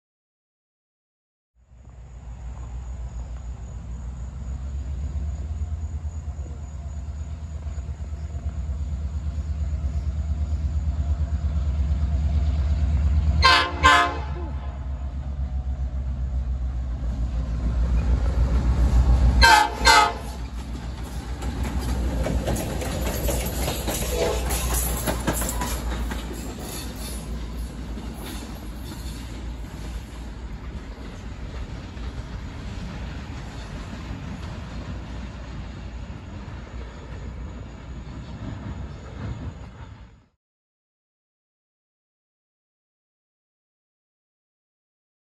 Buckingham Branch Excursion Train!
The Blue Ridge Flyer heads east to Ivy to turn around and head back to Stanton. The train is run by Buckingham Branch RR. 8-12-22

MP 194.13
Radio 160.455